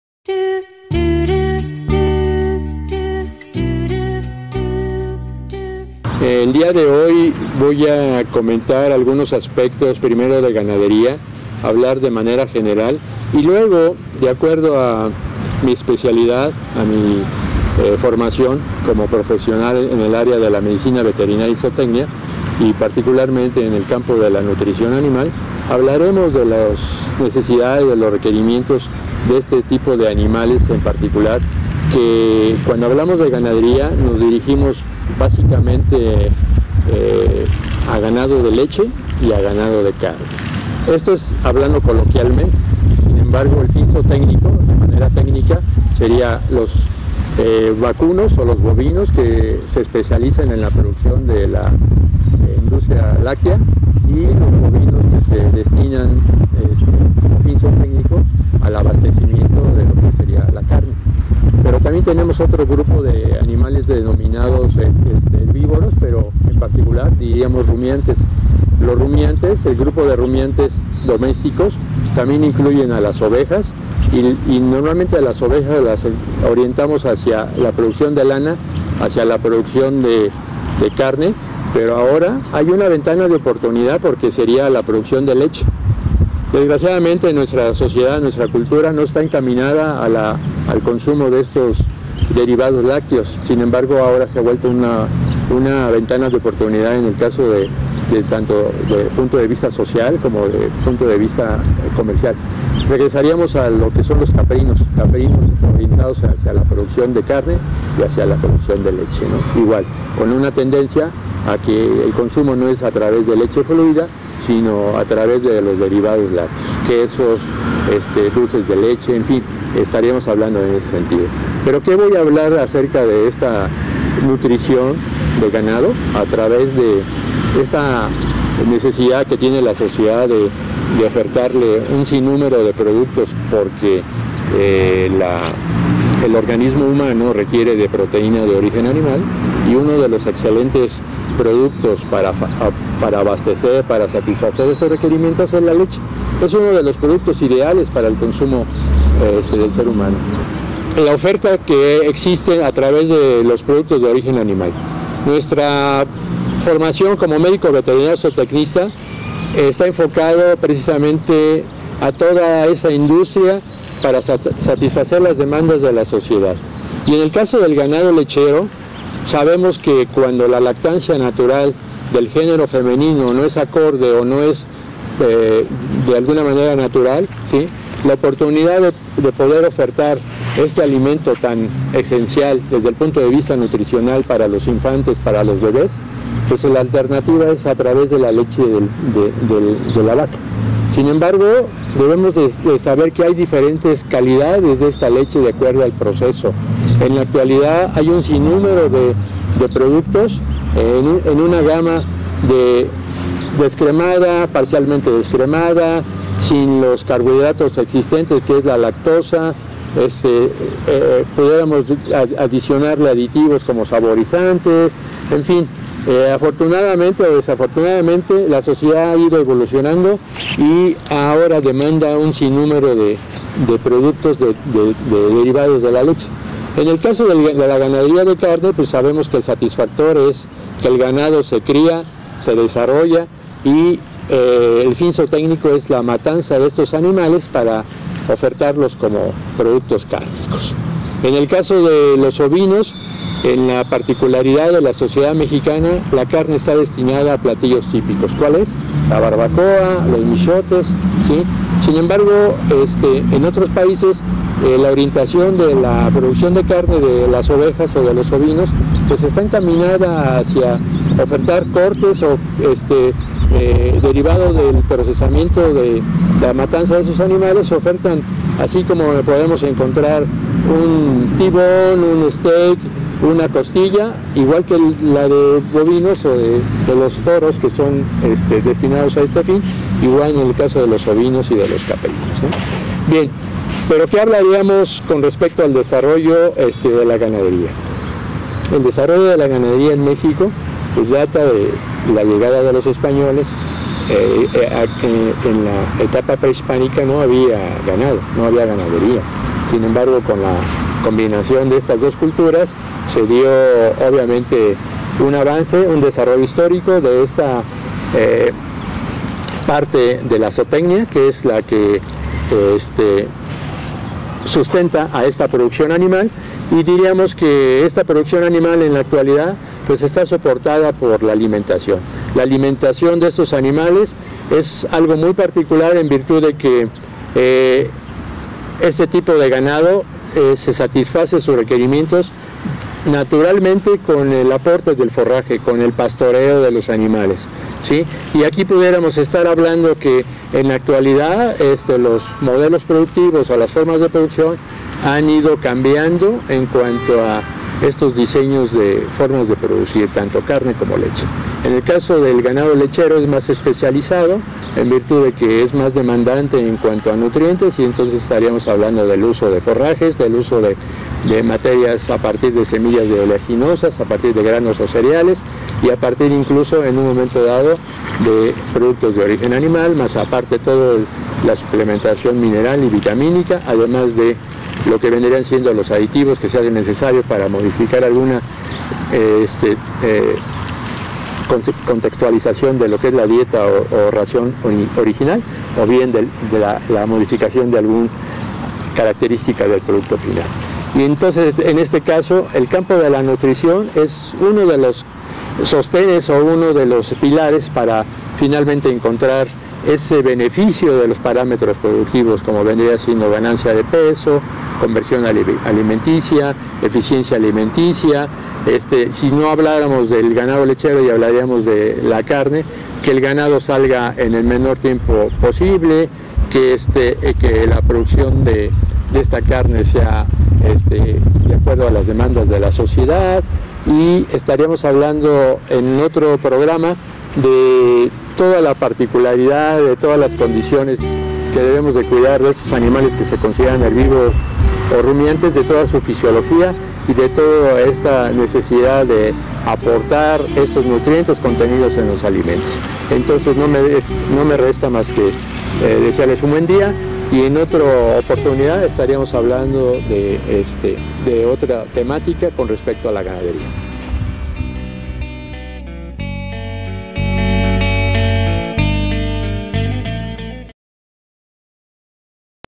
Generalidades Nutricionales en el Ganado Bovino por MVZ Sergio Carlos Ángeles Campos
El Fortalecimiento en el Quehacer de las Médicas y Médicos Veterinarios Zootecnistas en México ¡¡ Nos tiene acostumbrado el Dr. Sergio Ángeles a sus entrevistas sobre el cuidado en la Alimentación de las Pequeñas Especies, ahora acompañado de Theo nos comparte Generalidades Nutricionales en el Ganado Bovino , Ovino y Caprino siendo un conocedor en esta Materia de la Nutrición Animal .